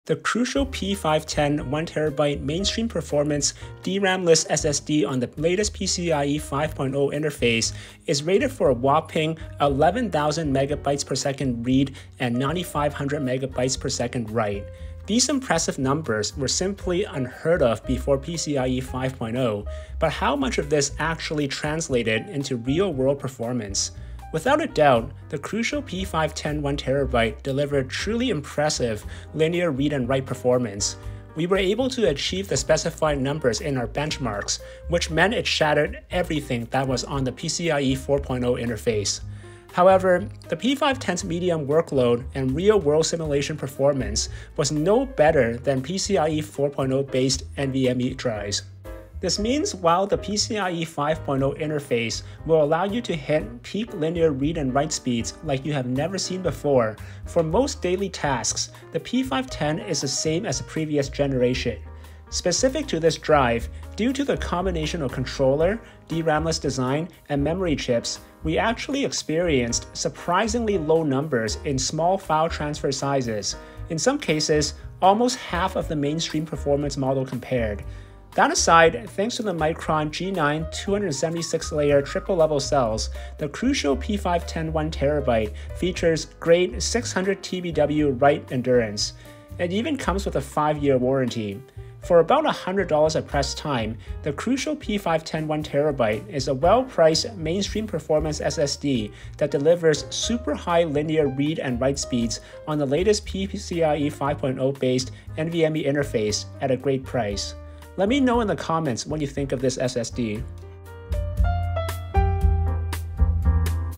11000MB/s for $100! Crucial P510 1TB PCIe 5.0 NVMe SSD Review
The Crucial P510 1TB mainstream performance SSD delivers extremely high linear read and write speeds on the latest PCIe 5.0-based NVMe interface at a great price, but its real world results are no better than the previous generation.